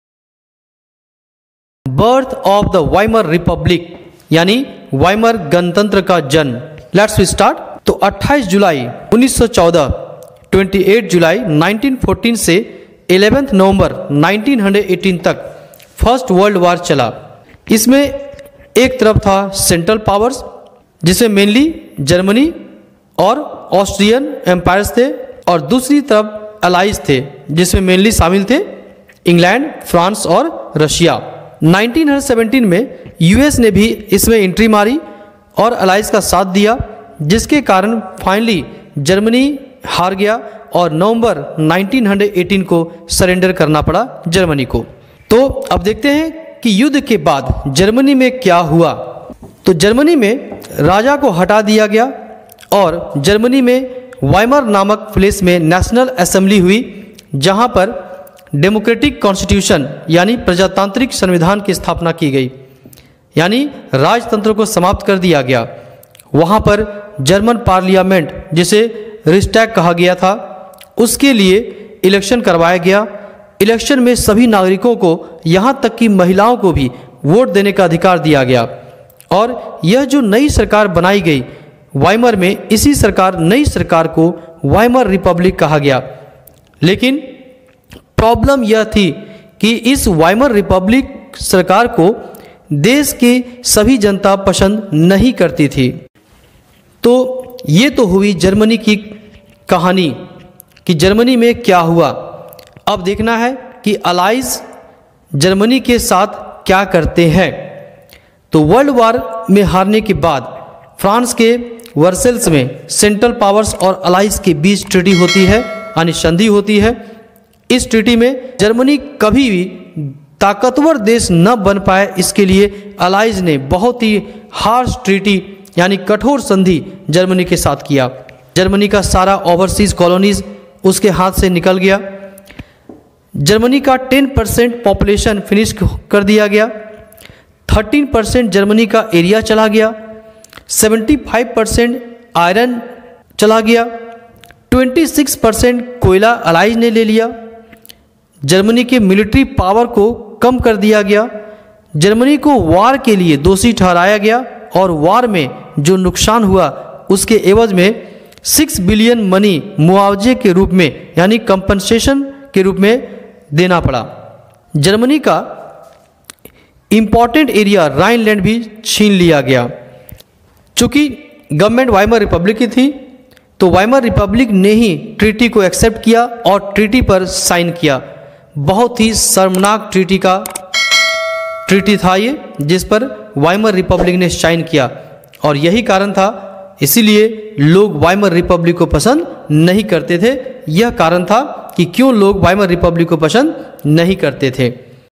Nazism and the Rise of Hitler / Part 2 / The Birth of Weimar Republic / Class 9 History By SRB/
Nazism and the Rise of Hitler / Part 2 / Birth of  the Weimar Republic / History/ class 9 / Chapter 3 /ncert/cbse / Explanation in hindi/ History By SRB

Germany fought the first world war in 1914 to 1918 alongside the Austrian empire and against the Allies England , France and Russia. US entered in 1917 and joined Allied powers.
The Peace Treaty at Versailles with the Allies was harsh and humiliating.